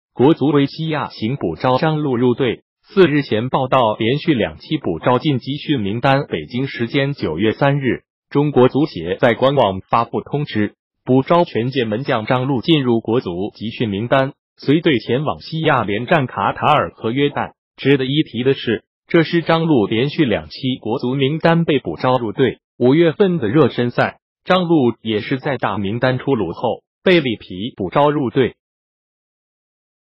國足為西亞行補招張鷺入隊！4日前報到 連續兩期補招進集訓名單
北京時間9月3日，中國足協在官網發布通知，補招權健門將張鷺進入國足集訓名單，隨隊前往西亞連戰卡塔爾和約旦。